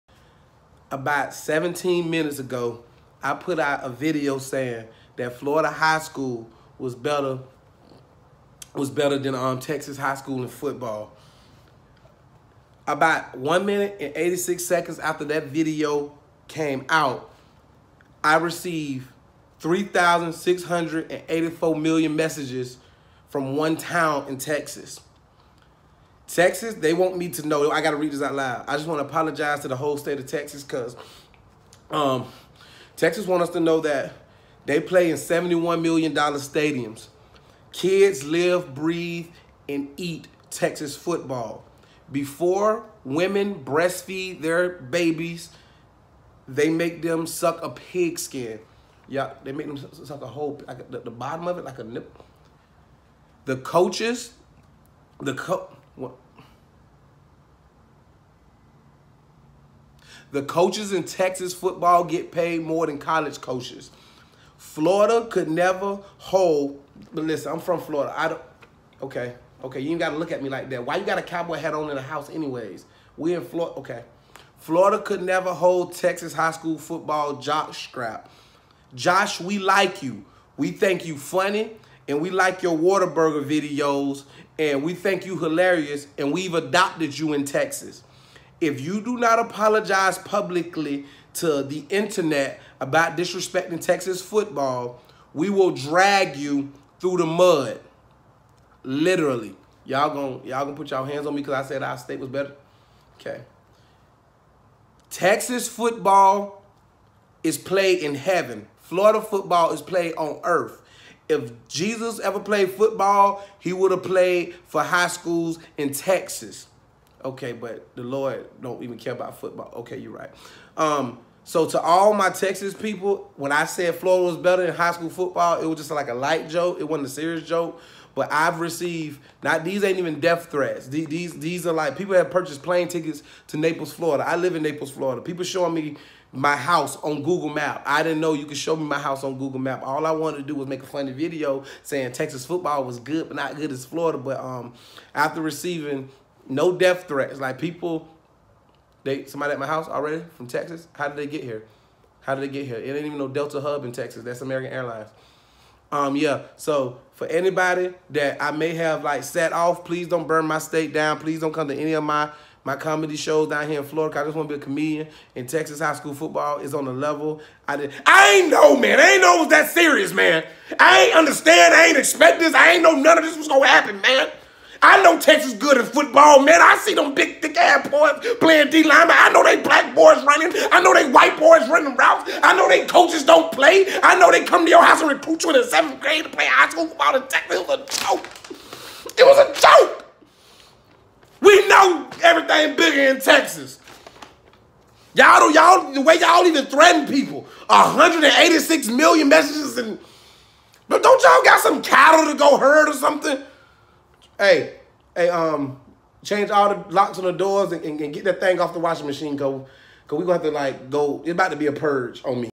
Corrections, TEXAS HIGHSCHOOL FOOTBALL IS THE BEST IN HISTORY
Comedian Josh Pray was corrected by 111,111,100,007,564,737 Texans ...seriously